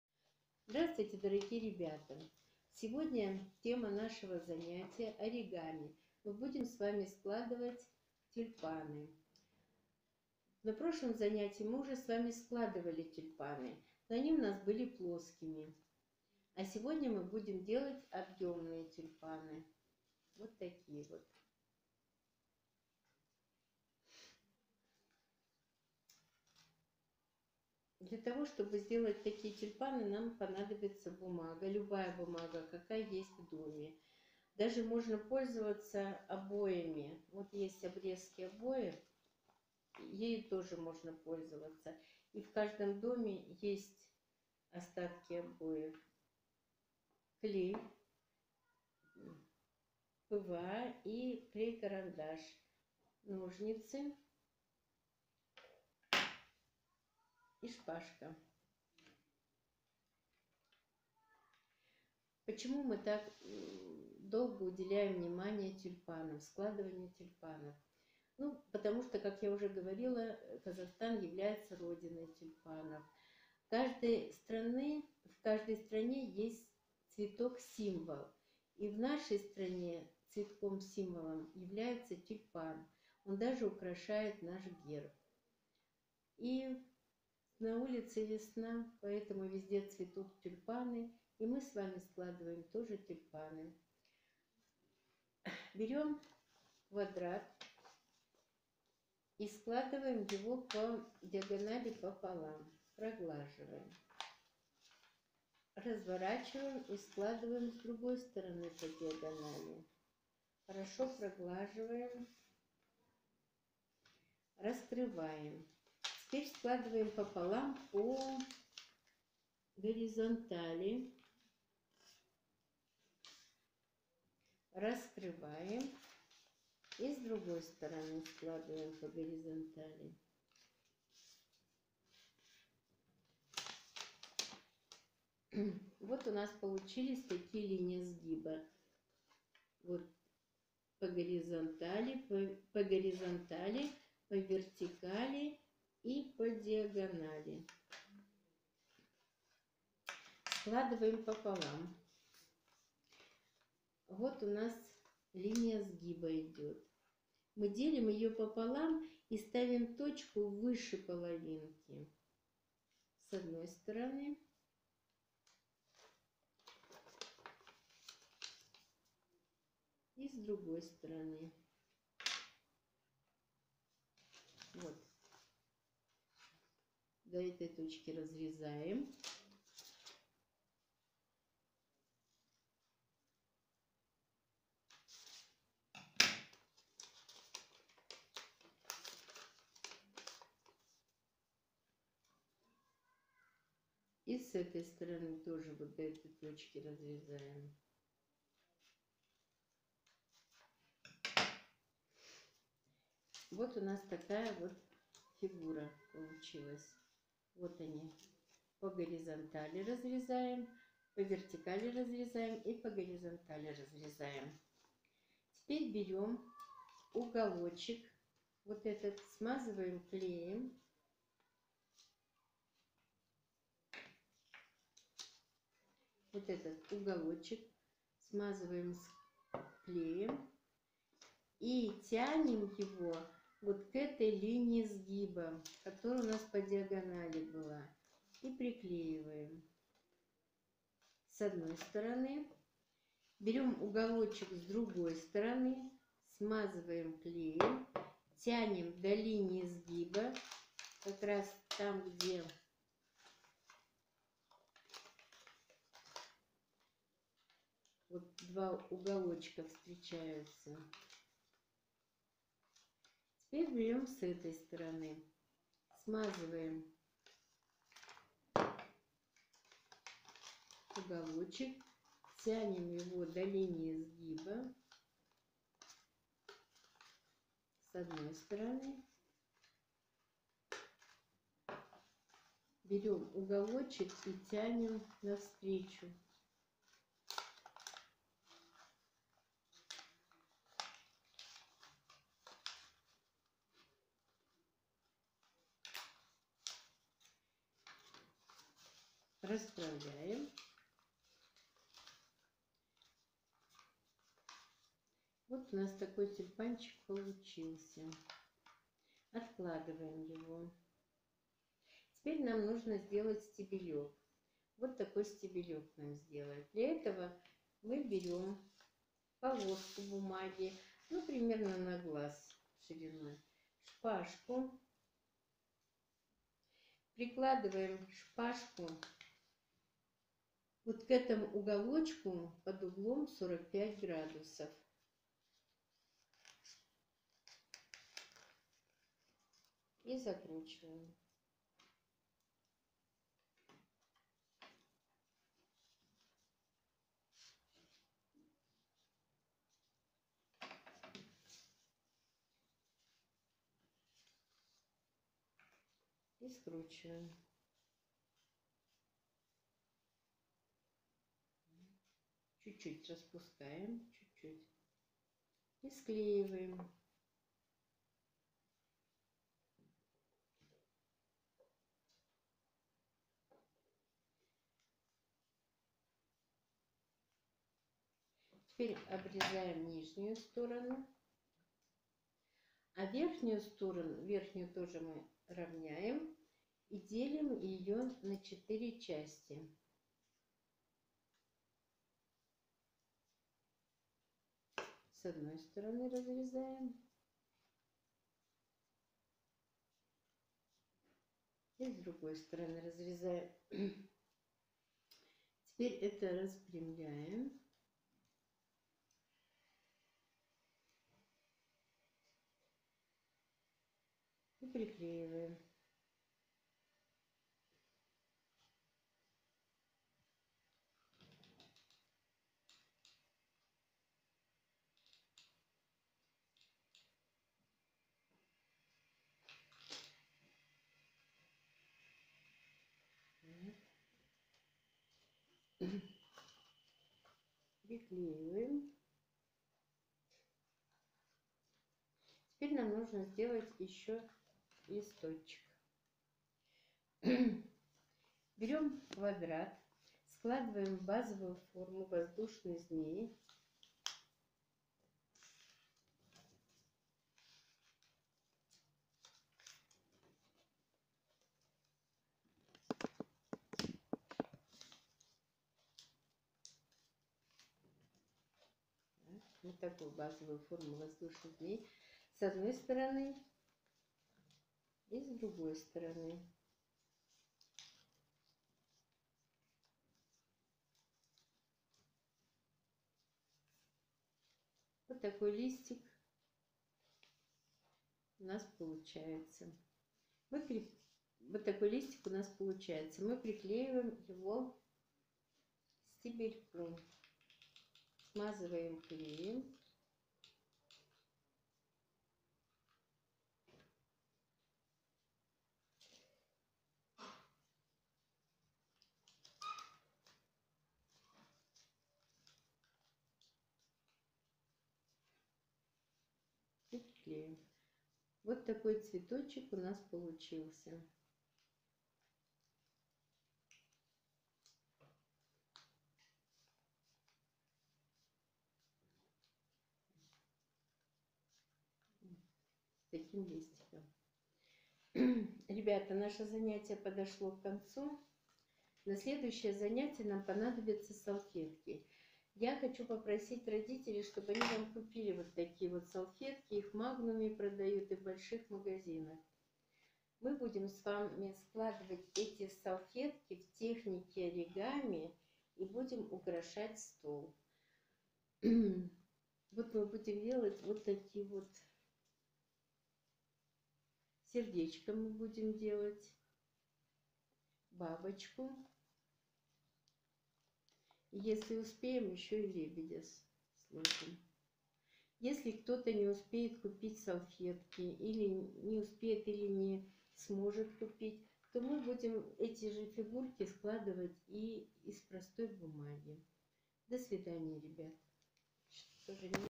Урок 32 - Оригами. Тюльпаны.
Дворец школьников города Алматы, эколого_биологическое объединение "Юная мастерица".
Педагог дополнительного образования.   Беседина Л. А.
Тема занятия :"Оригами. Тюльпаны.
Цели: развивать мелкую моторику,т пространственное мышление и умение сконцентрироваться.